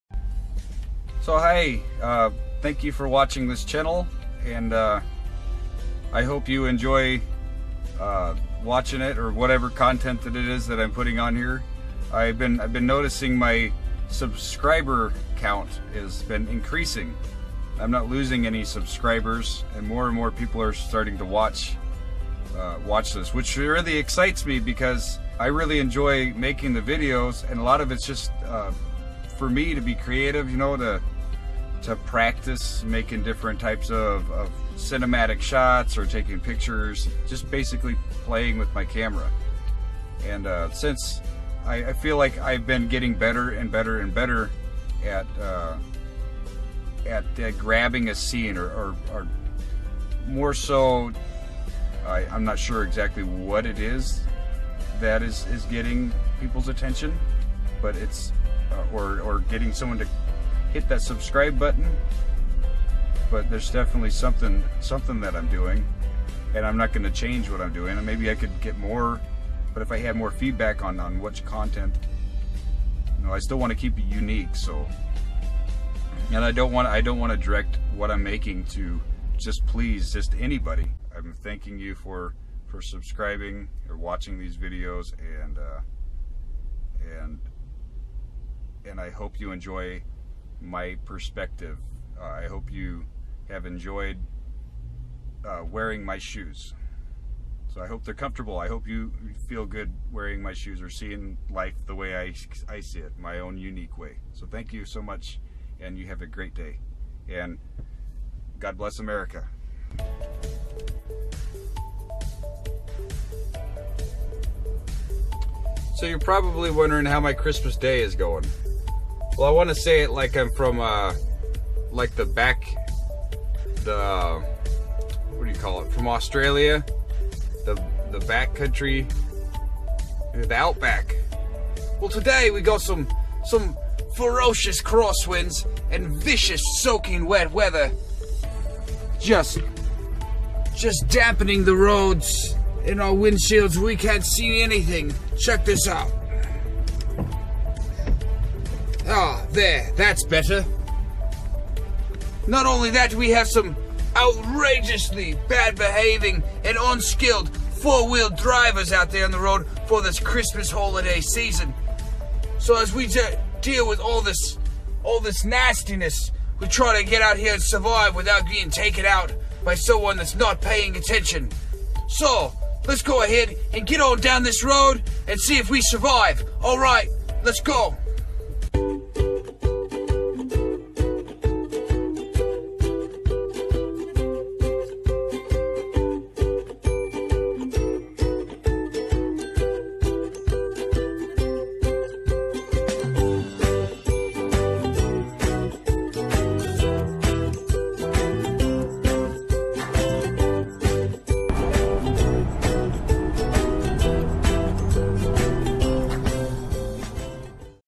Thank You For Participating
I appreciate that subscription and the views. I plan on continuing to film and take pictures of my travels around the country as well as sharing my life and views through YouTube. It's motivating me to purchase a DSLR camera for making cinematic quality vlogs.